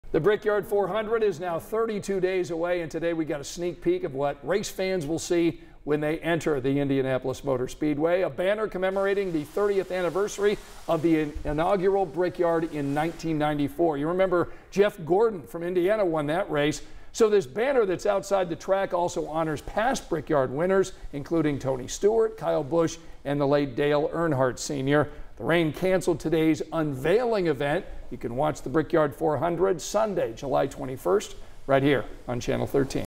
Banner at IMS entrance pays tribute to three decades of Brickyard champions
The Brickyard 400 is just 32 days away.